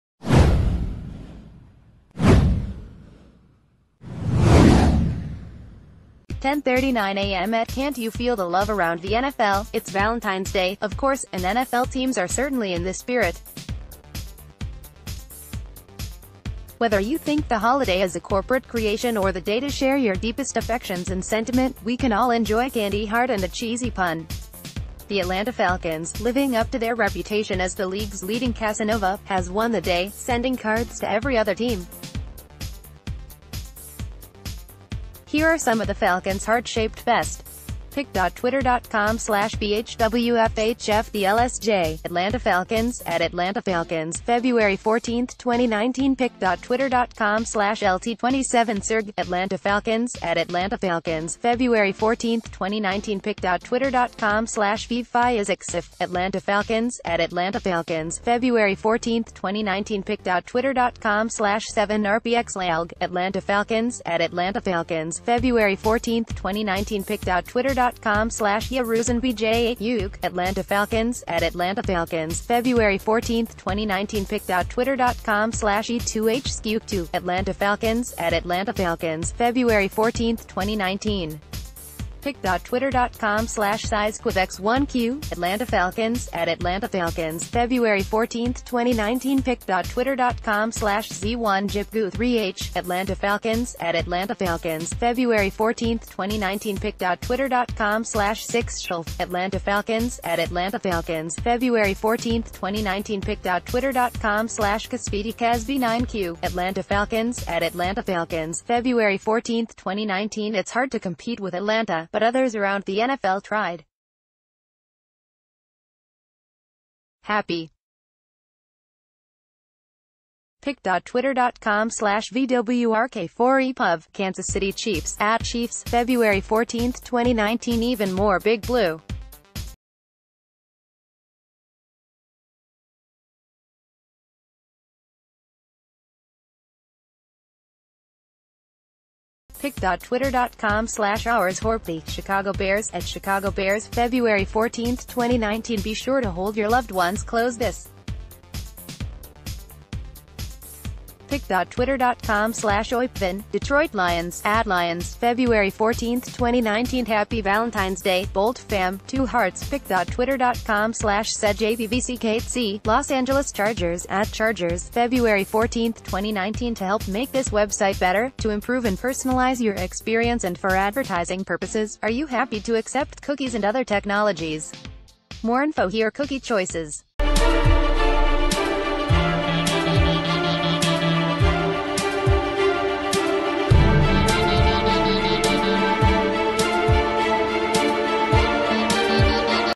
NFL teams send their Valentines
10:39 AM ETCan't you feel the love around the NFL?It's Valentine's Day, of course, and NFL teams are certainly in the spirit. Whether you think the holiday is a corporate creation or the day to share your deepest affections and sentiment, we can all enjoy a candy heart and a cheesy pun. The Atlanta Falcons, living up to their reputation as the league's leading Casanova, has won the day, sending cards to every other team. Here are some of the Falcons' heart-shaped best.